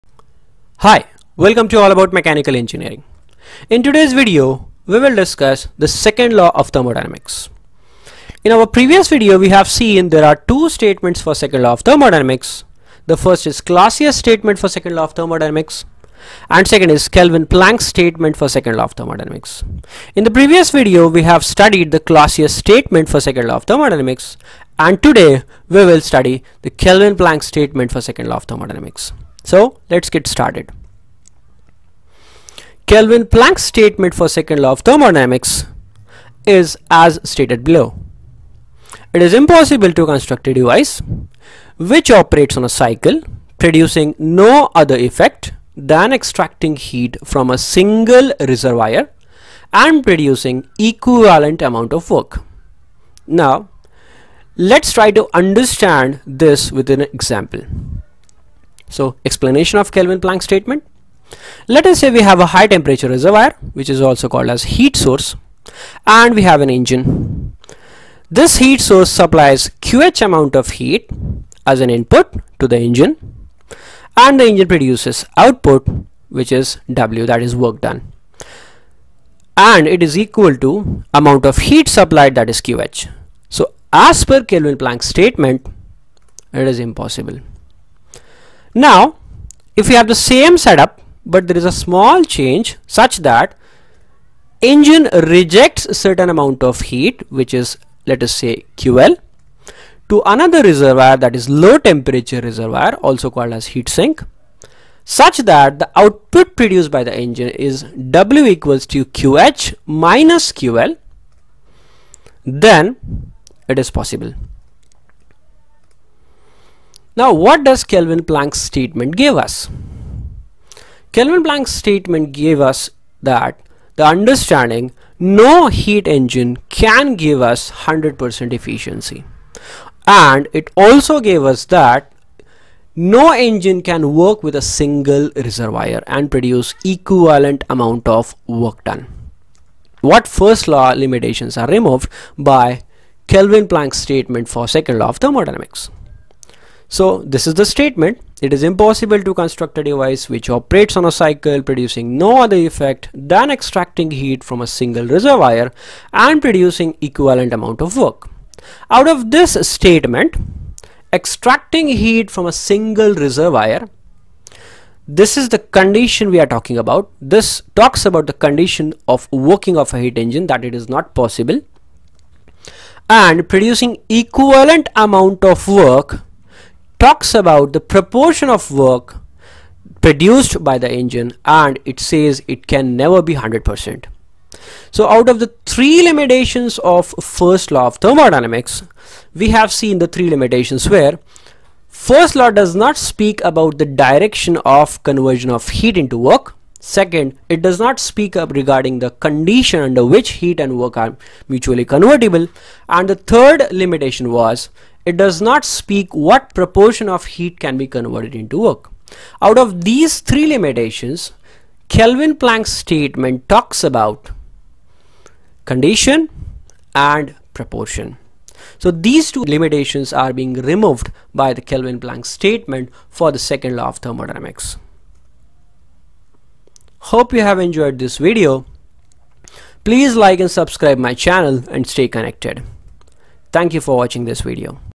Kelvin Planck Statement for Second law of Thermodynamics
Kelvin plank statement of thermodynamics | kelvin plank statement of second law of thermodynamics
Short Video explaining Kelvin-Planck Statement for second law of thermodynamics.